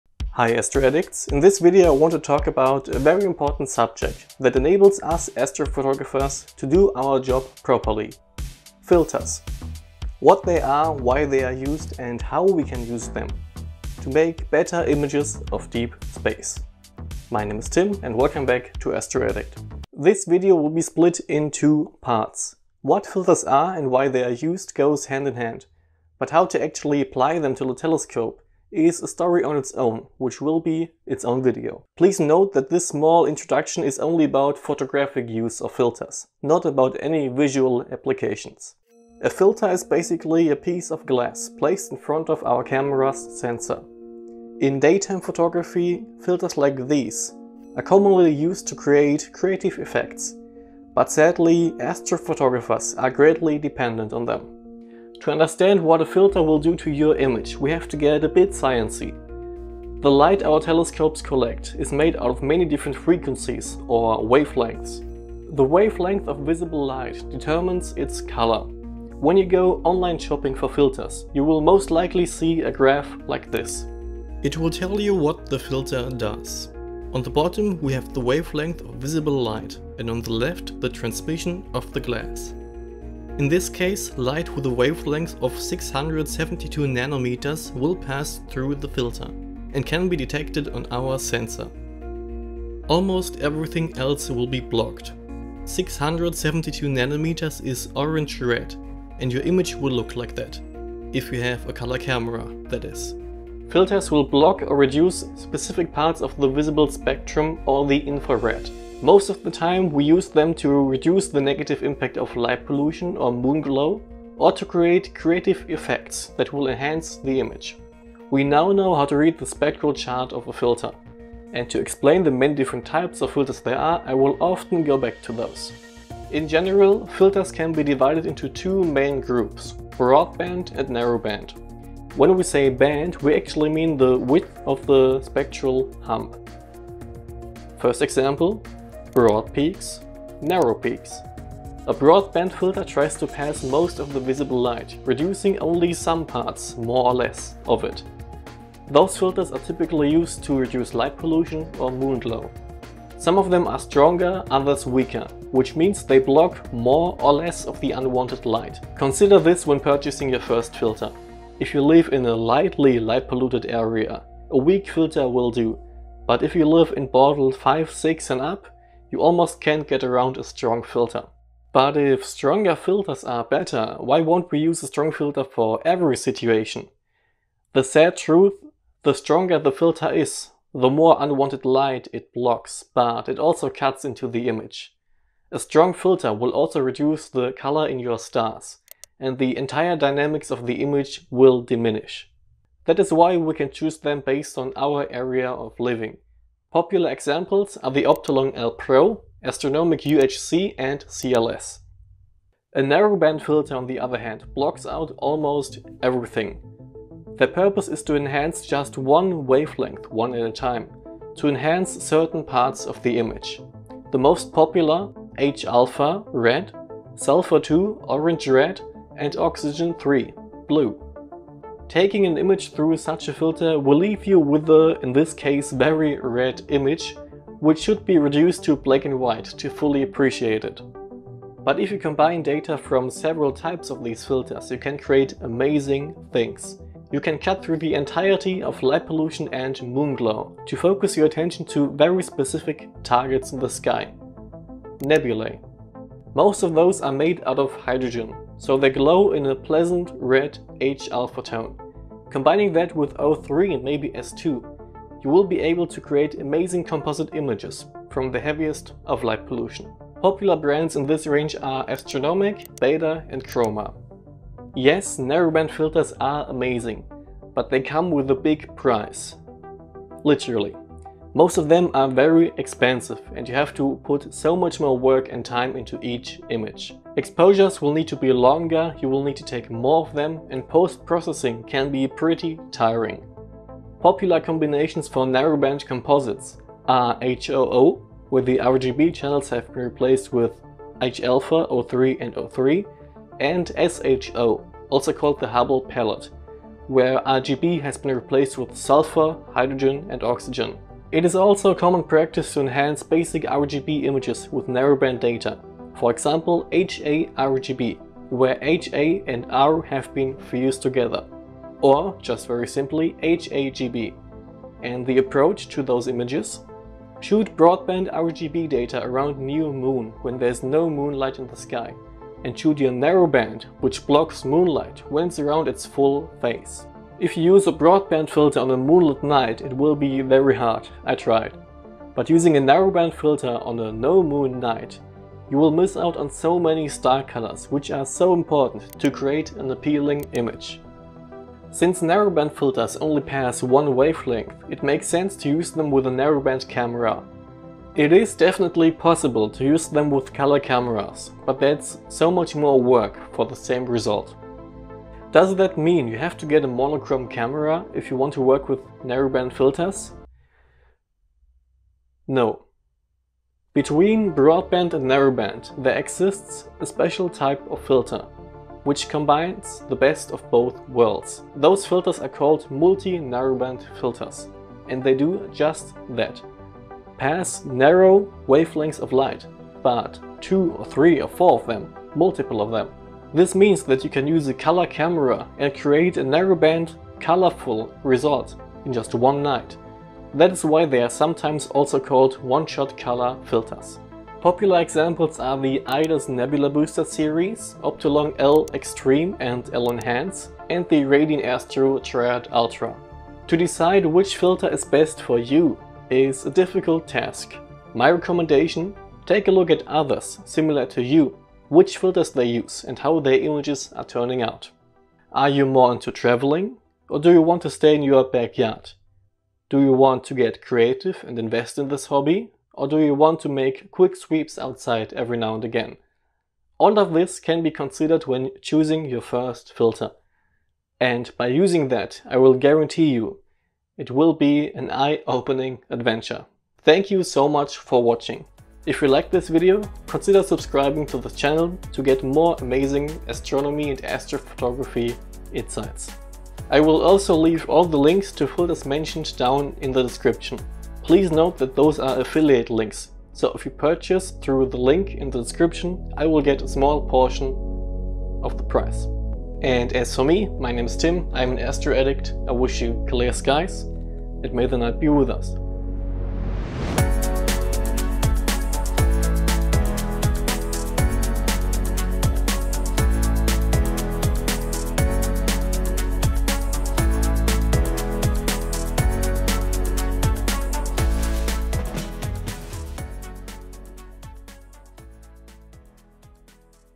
My Filters For Astrophotography
Welcome back AstroAddicts!
I often get questions about which filters to use in which situation. That's why i decided to make a video on that topic, and answer the most basic questions regarding filters!

All content in this video was either filmed by myself, spoken by myself, taken by myself. Exceptions:
Images of several filters taken from respective websites

Thank you so much for stopping by, i wish you a wonderful day/night wherever you are, clear skies, and

May the night be with us!